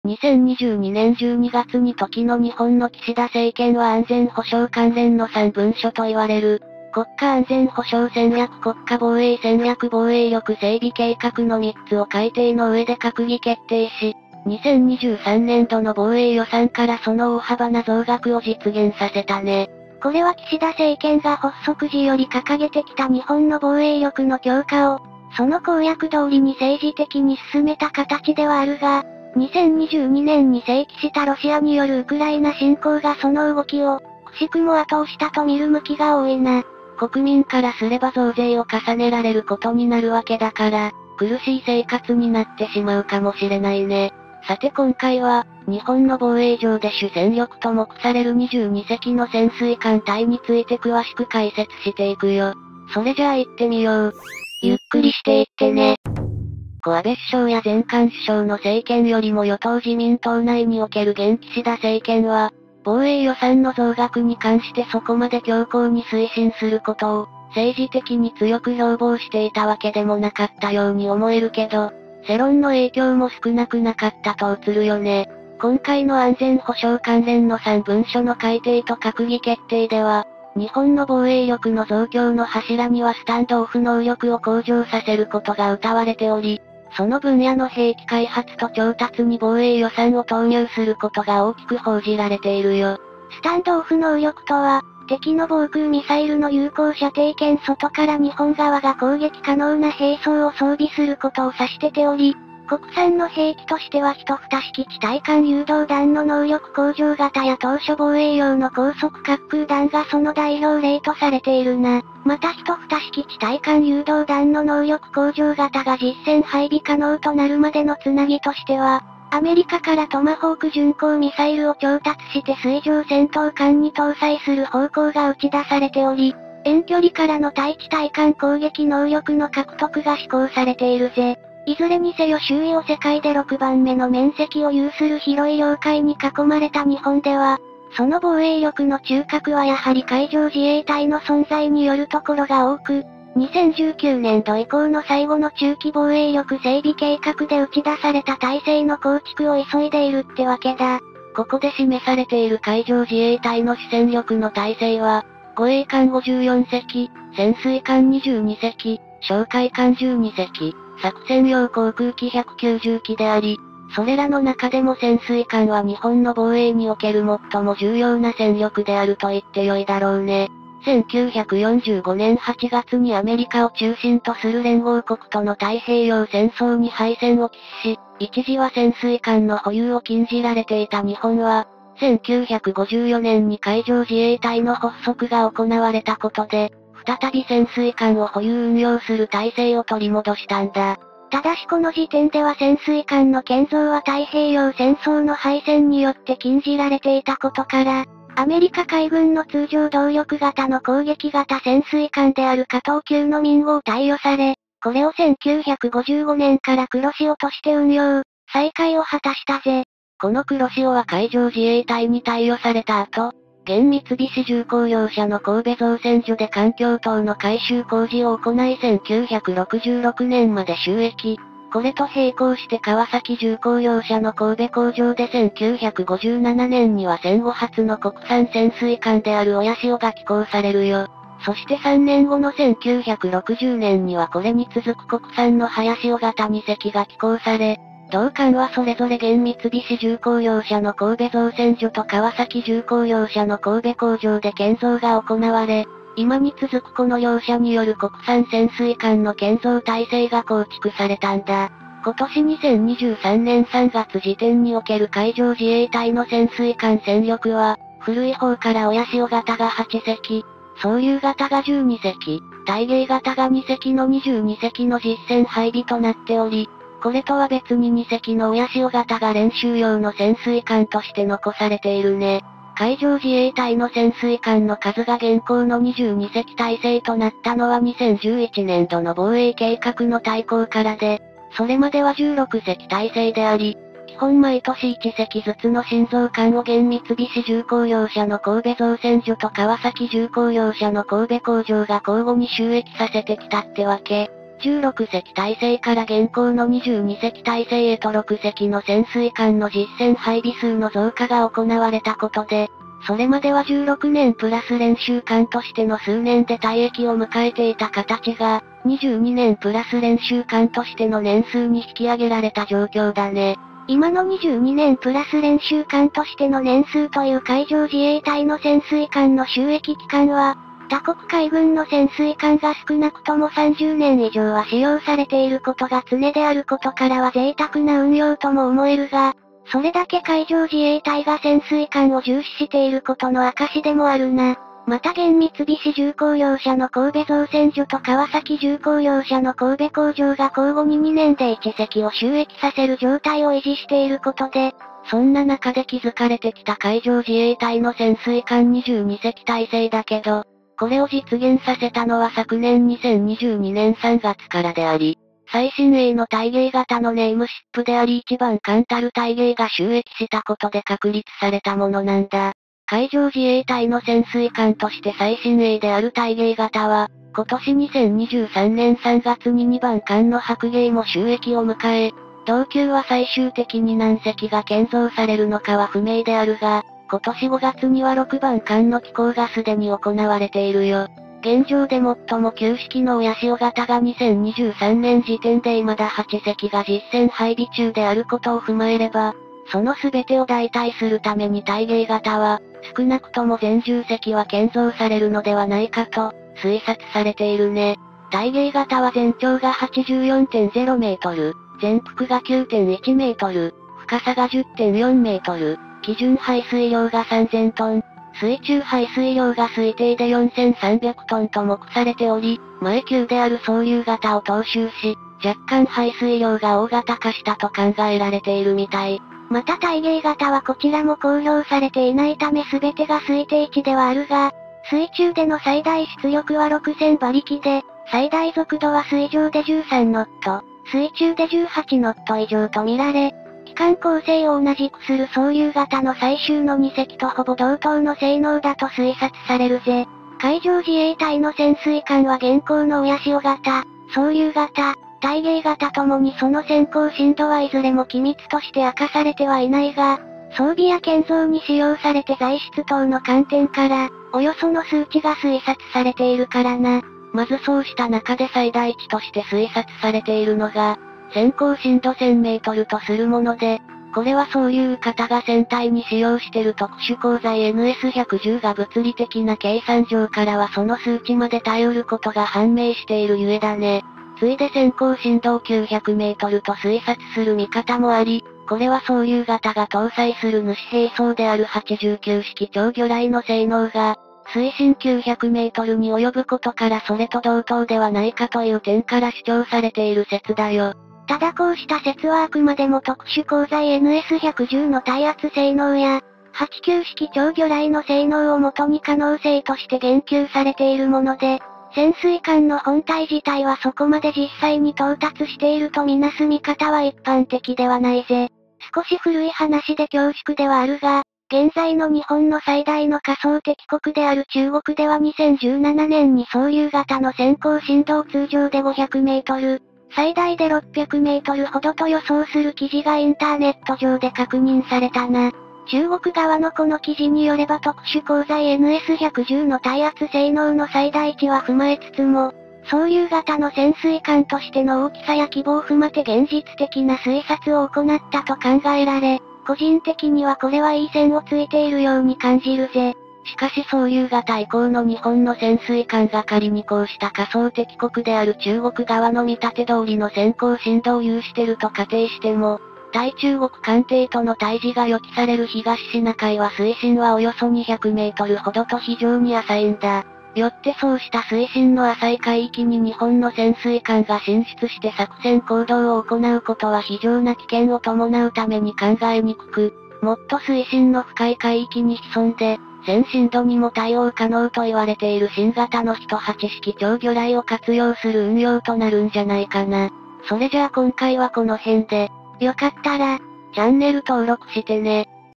【ゆっくり解説・軍事News】海自潜水艦ついに潜航深度バレる特殊鋼材NS110と89式超魚雷で900mか！三菱川重建造最強潜水艦の静粛性に国連ビビる？
海自潜水艦ついに潜航深度バレる特殊鋼材NS110と89式超魚雷で900mか！三菱川重建造最強潜水艦の静粛性に国連ビビる？

引用・転載「出典：海上自衛隊ホームページ」
引用・転載「出典：航空自衛隊ホームページ」
引用・転載「出典：陸上自衛隊ホームページ」